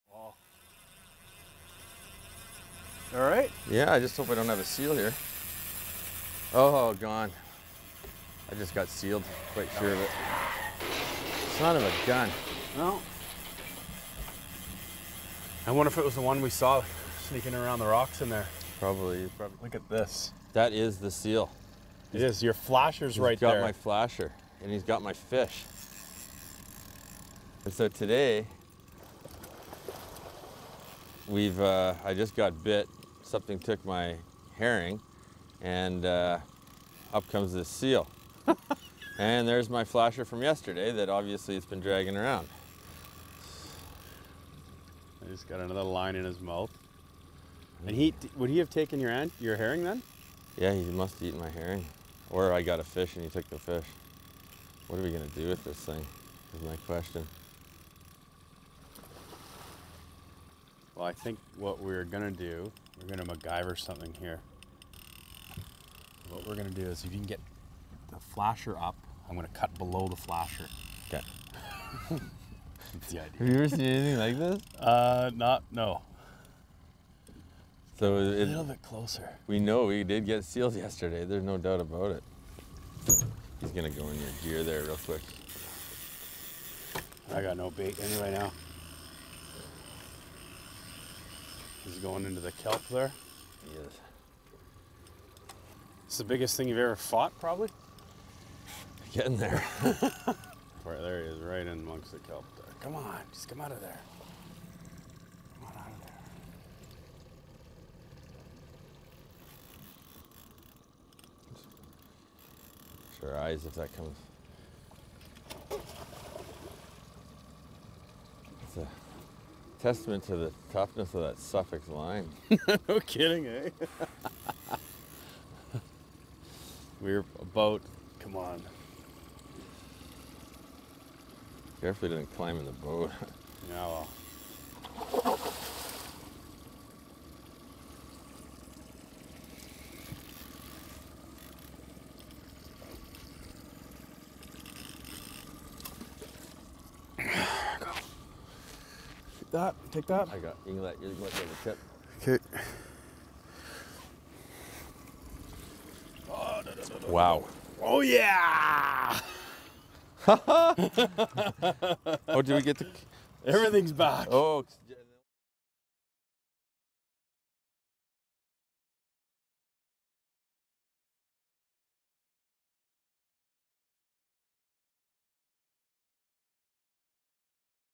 Seal takes Fishermen's Fish and Bait two days in a row
When filming an episode in Port Hardy, we discovered a seal was taking our fish two days in a row and had then got caught in our bait. We released the seal straight away and there was no harm to the seal.